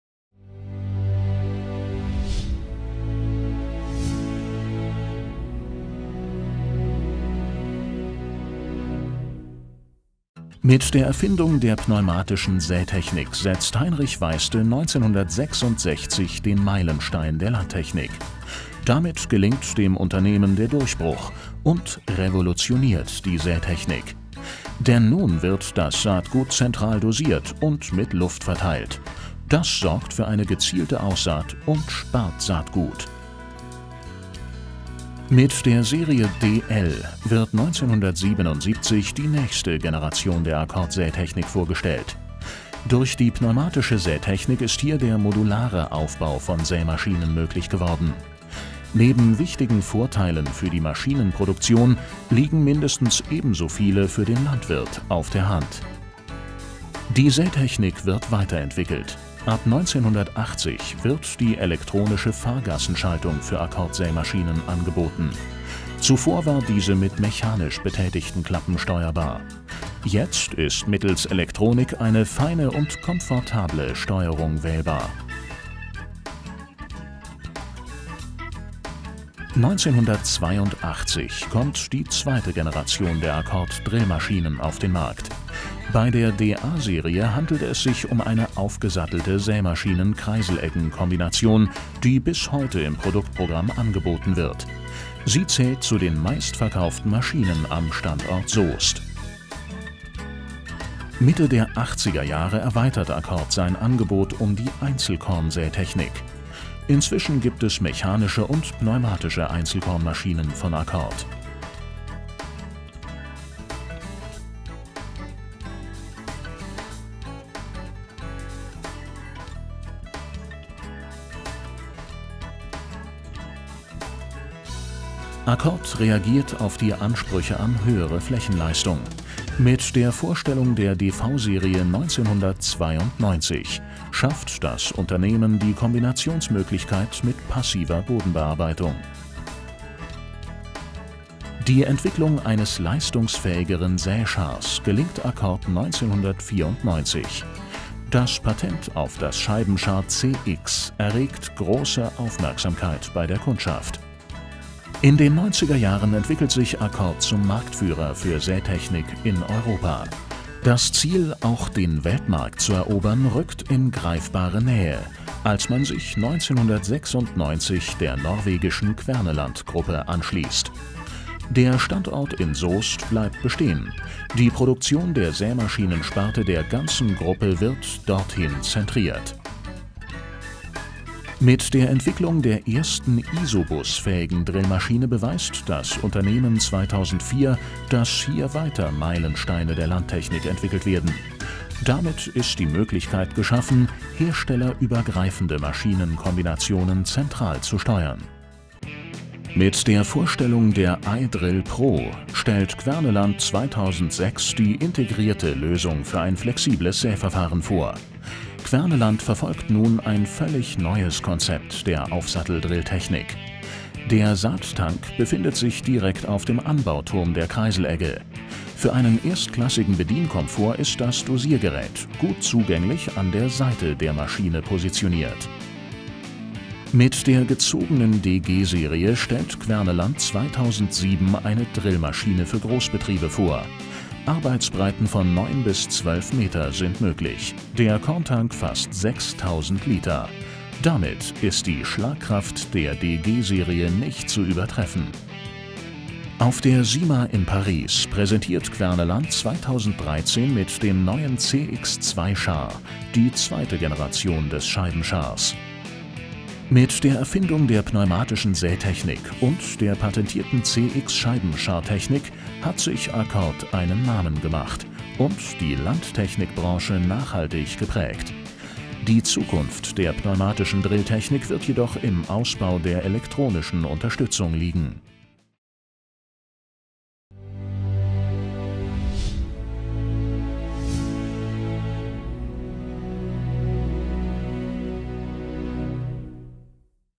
Pneumatische Drilltechnik - ein Meilenstein der Landtechnik
Wer sich heute über die Entwicklung der Sätechnik informiert wird sehr schnell über den Namen Accord stolpern und feststellen, dass sich hinter dem Namen weitaus mehr als ein umfassendes Programm an international anerkannten Sämaschinen steht.